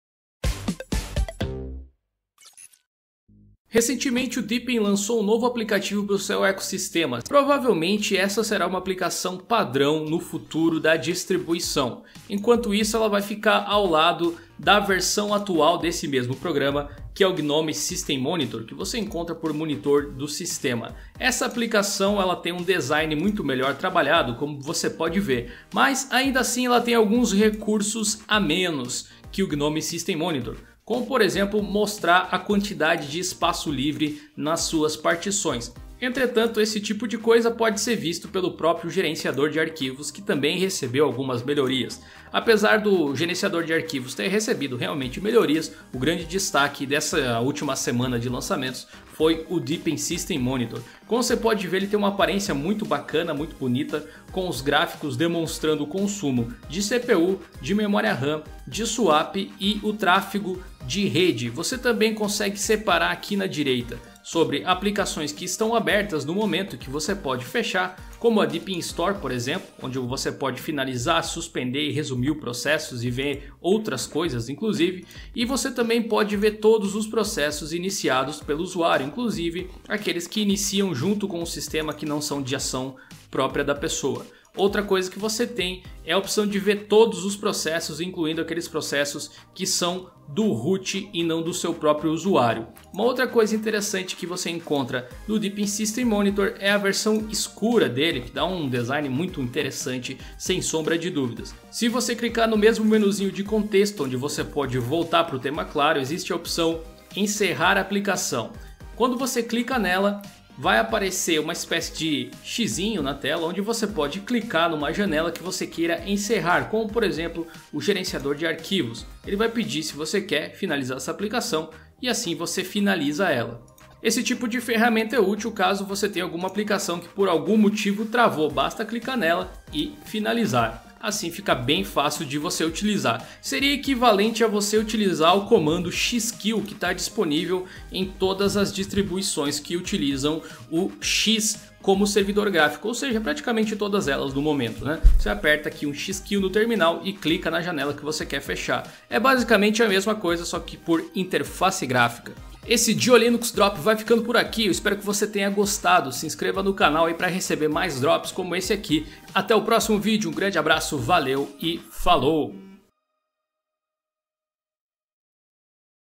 Conheça o incrível Deepin System Monitor - Diolinux Drops
O Deepin vem caprichando nos detalhes, a nova aplicação da "família Deepin" é o Deepin System Monitor, uma ferramenta para você fazer a leitura de informações do sistema, como consumo de processador, memória, SWAP, rede e processos do sistema, com recursos para finalizar janelas e suporte a temas.